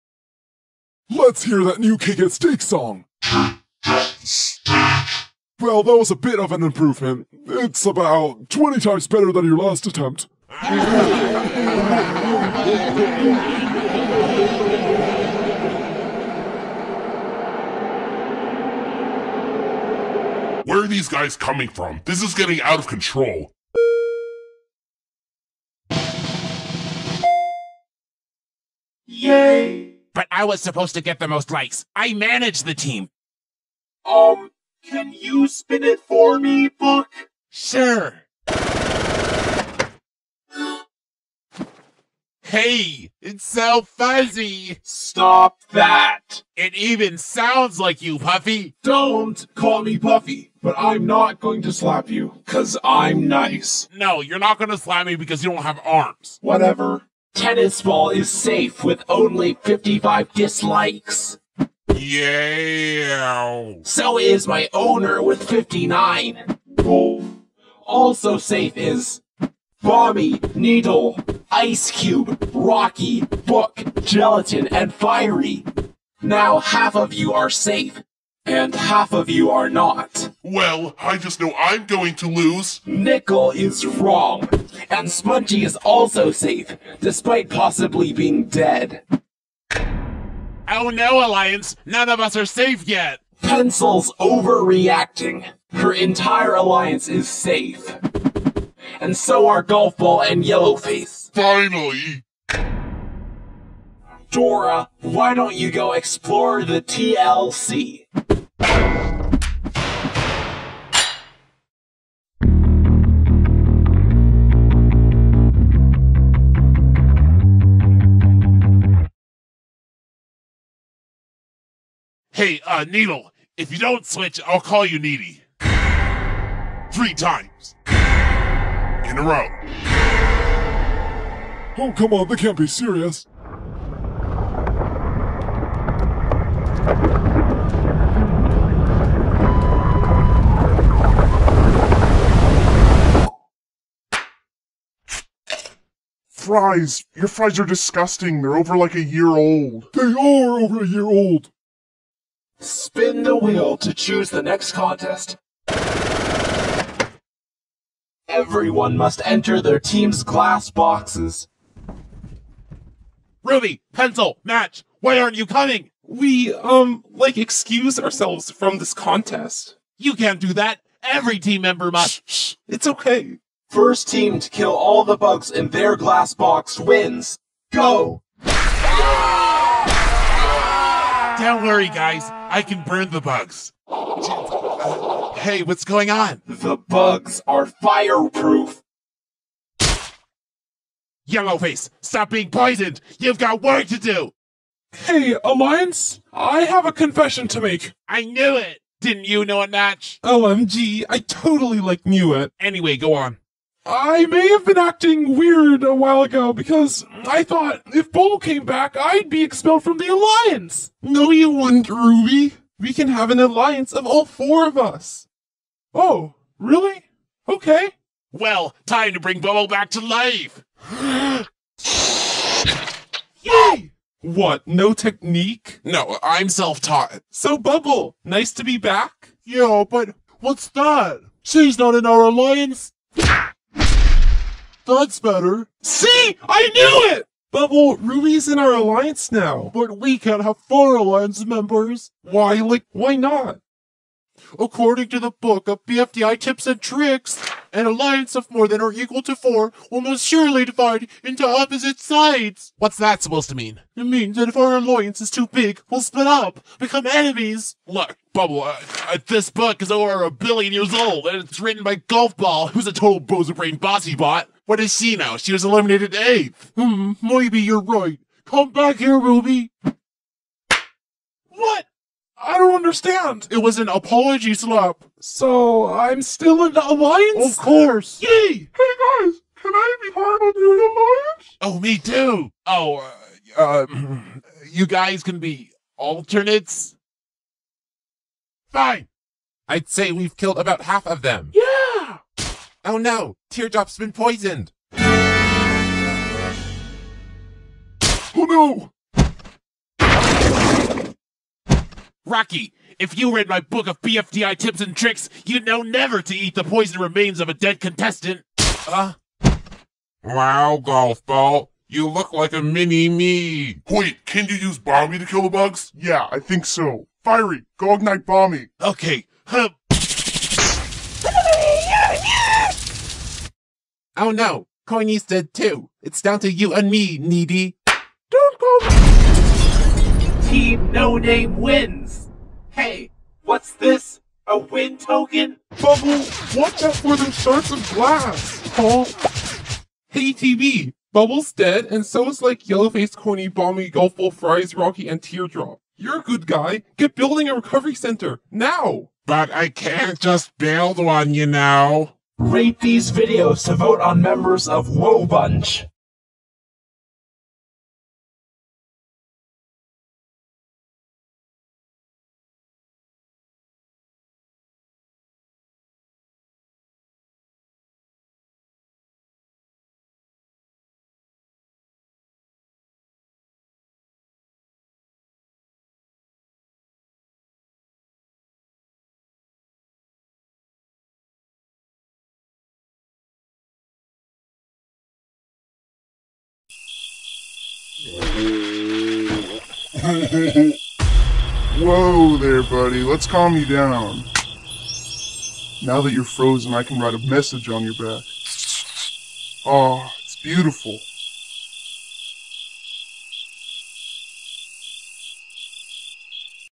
BETA 3: Bugs Bugs And More Bugs
Story:
The eliminated object is dora. The two teams. Team W.O.A.H bunch and Team No Name Are Going To Fight Again. Team WOAH bunch loses and are up for elimination.